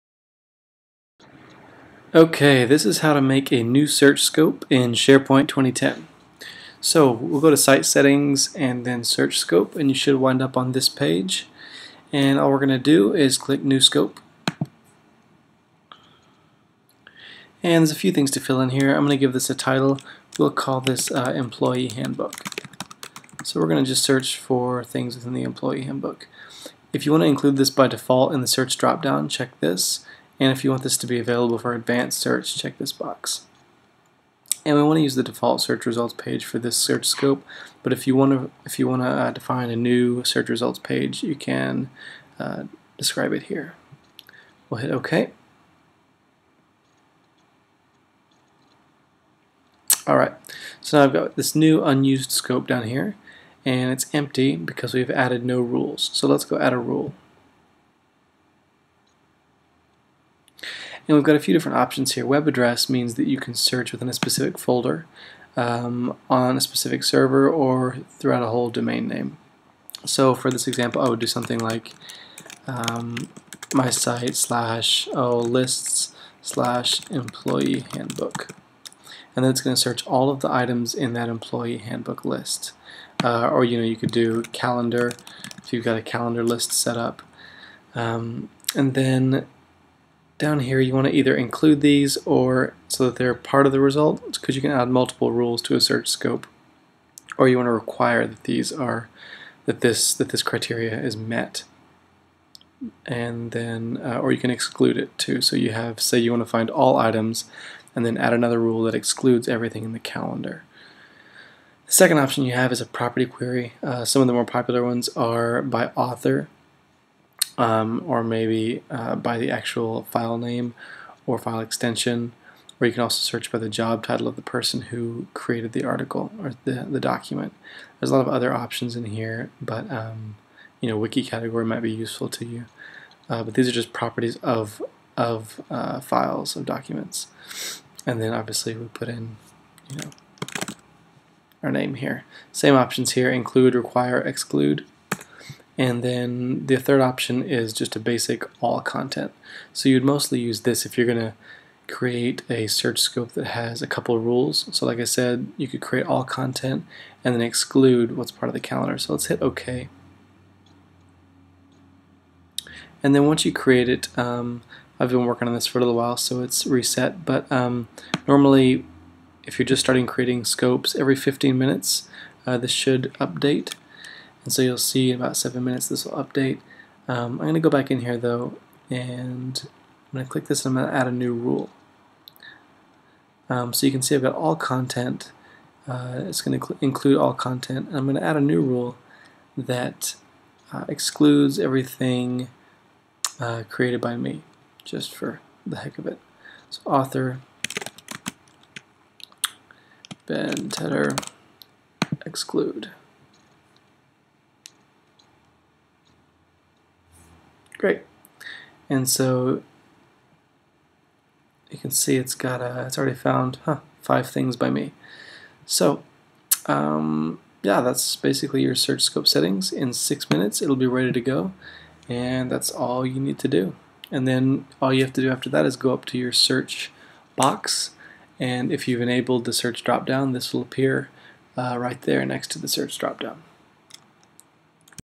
SharePoint 2010 Custom Search Scopes
Create a custom search scope in SharePoint 2010. Add and remove rules to your search scope for ultimate flexibility.